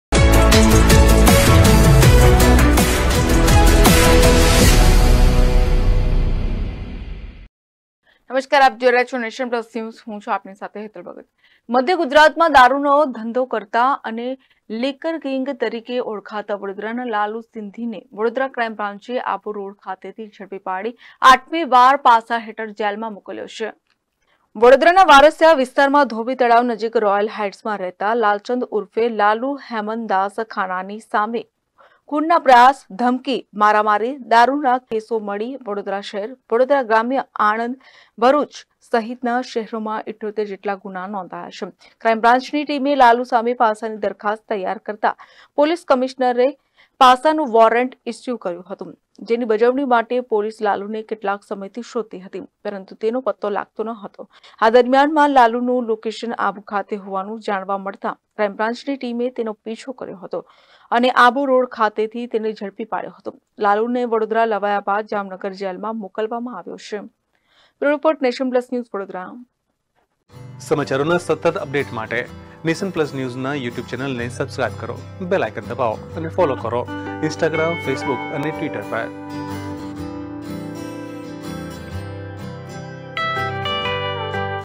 લાલુ સિંધી પાસામાં ધકેલાયો
મધ્ય ગુજરાતમાં દારૃનો ધંધો કરતા અને લિકરકિંગ તરીકે ઓળખાતા વડોદરાના લાલુ સિંધીને વડોદરા ક્રાઇમ બ્રાન્ચે આબુ રોડ ખાતેથી ઝડપી પાડી આઠમી વાર પાસા હેઠળ જેલમાં મોકલ્યો છે.

Vadodara's Lalu Sindhi, Liquor King liquor dealer Central Gujarat,arrested Vadodara Crime Branch Abu Road sent jail eighth time.